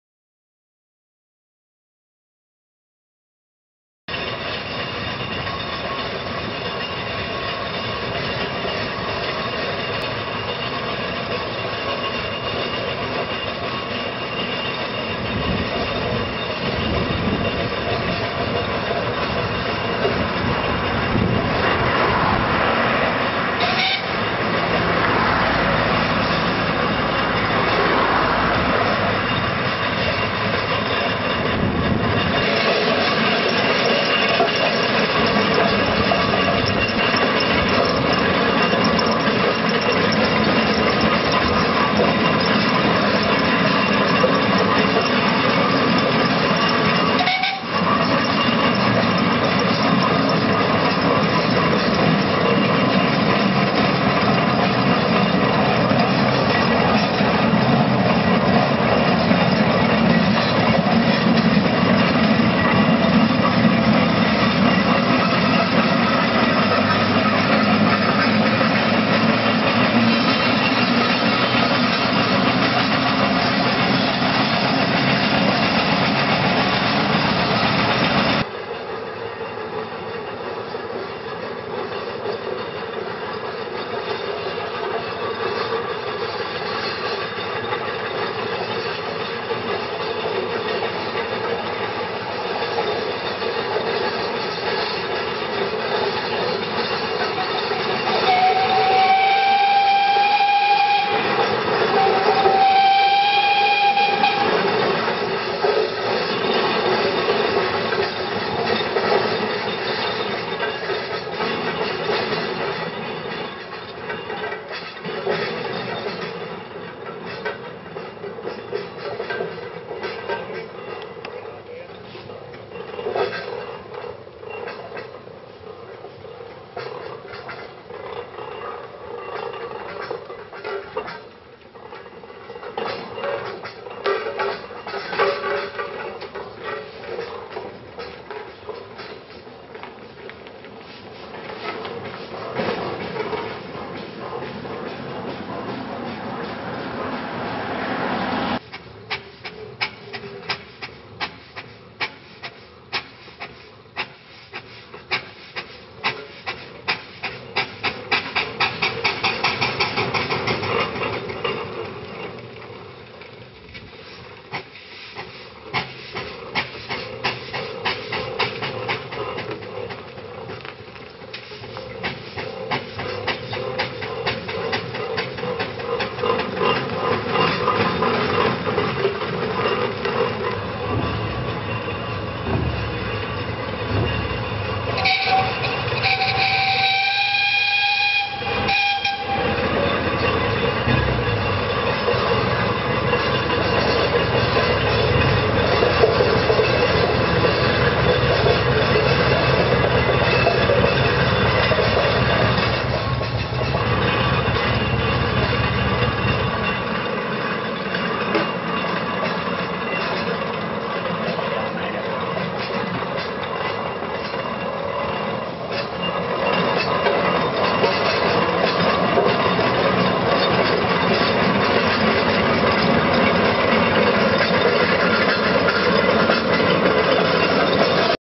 Aveling & Porter Compound 8 Ton Road Roller No. 8753
After years of battling with the elements, a former roller from the collection of the late Roy Ward, a familiar face on the rally fields in the 1960's-70's, was been purchased by Matt and Dan Barklem for preservation in October 2000. The Aveling & Porter Roller Works No. 8753 "Silver Streak" was rallied for many years in the local area by Roy. Although recovered a reasonable condition, the restoration is no mean task.